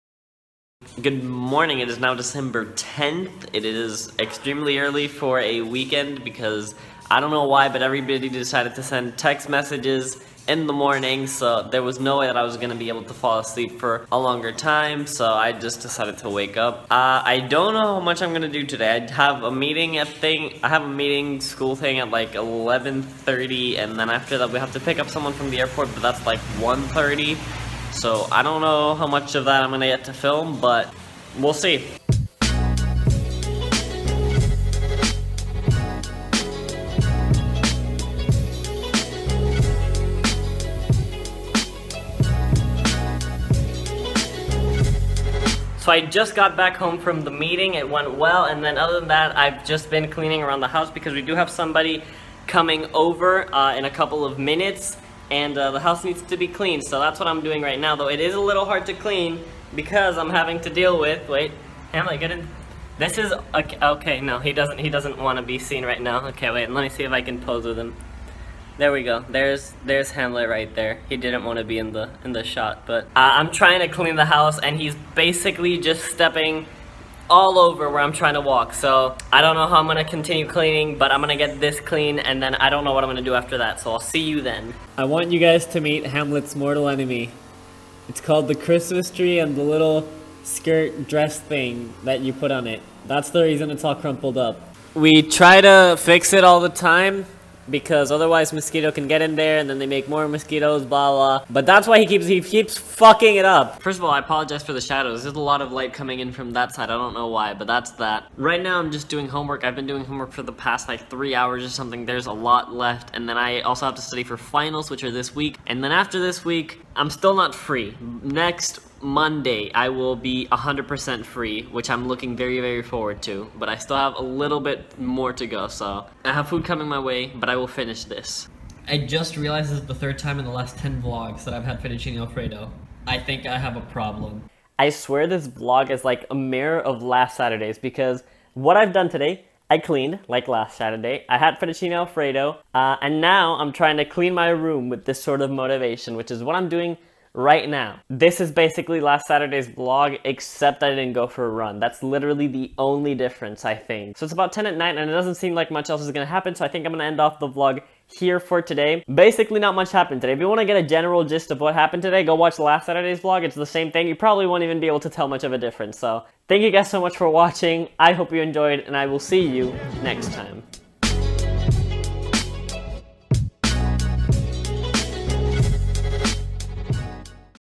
Hamlet, Get In The Shot (Day 10)
Welcome! Had a better day than yesterday thankfully. Not much happened today and I didn't realize until I started editing how many times I showed Hamlet in the vlog. Thank you guys so much for watching, I hope you enjoyed!